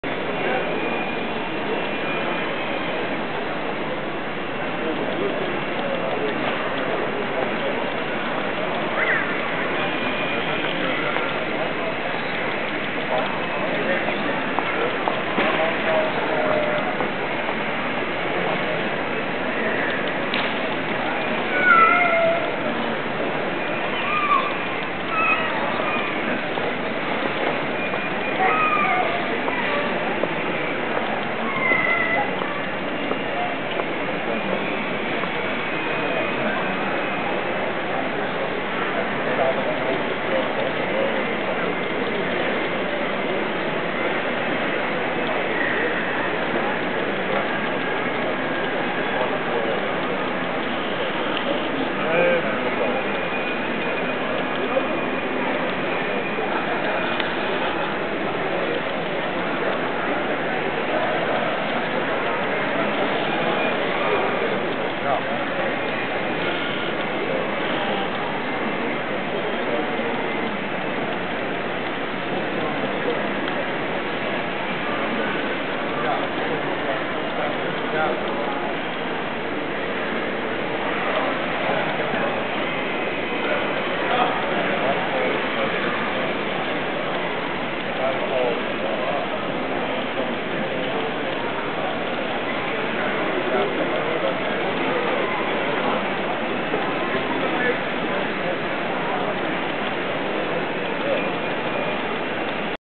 Modelbouw modeltrucks RC Trucks Scania Volvo Mercedes RC R.Hendriksma
RC Vrachtwagen wordt geladen door een shovel.
Op de modelbouwbeurs te Leeuwarden.